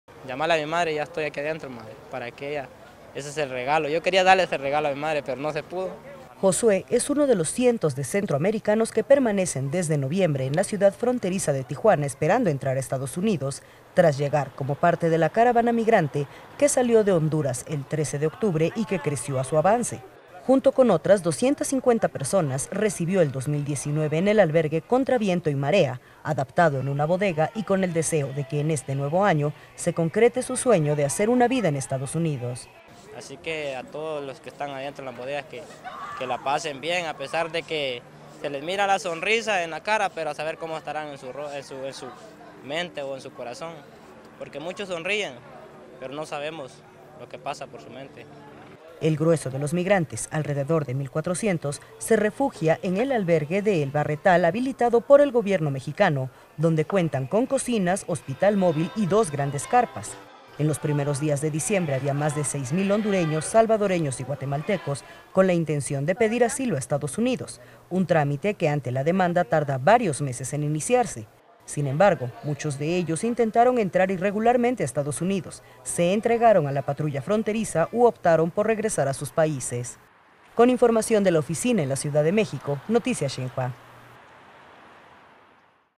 Migrantes de caravana reciben 2019 esperando cumplir su sueño
Josué es uno de los cientos de centroamericanos que permanecen desde noviembre en la fronteriza ciudad de Tijuana esperando entrar a Estados Unidos, tras llegar como parte de la caravana migrante que salió de Honduras el 13 de octubre y que creció en su avance.